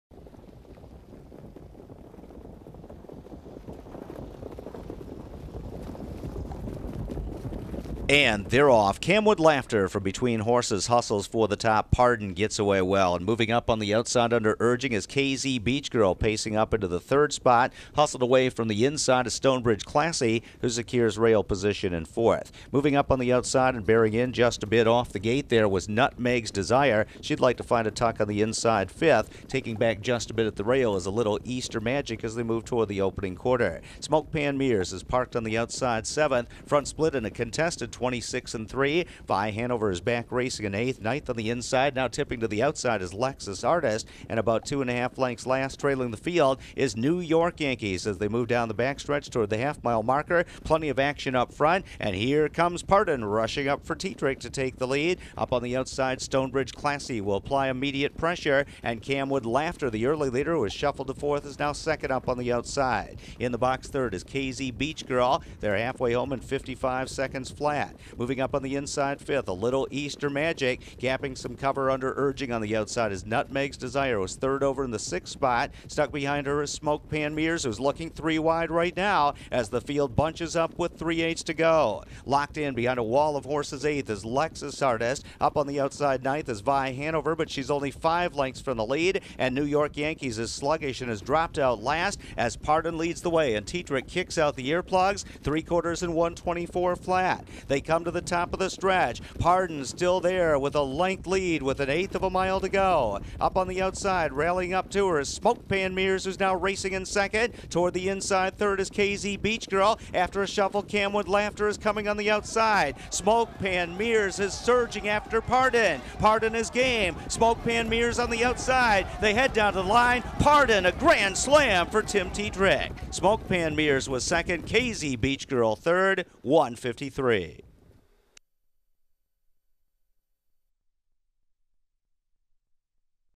March 7, 2014 - Race 10 - Pardon
Fillies & Mares C-2 - 1:53 - Driver: Tim Tetrick - Trainer: Linda Toscano
2nd: Smoke Pan Mirrors 3rd: Kz Beach Girl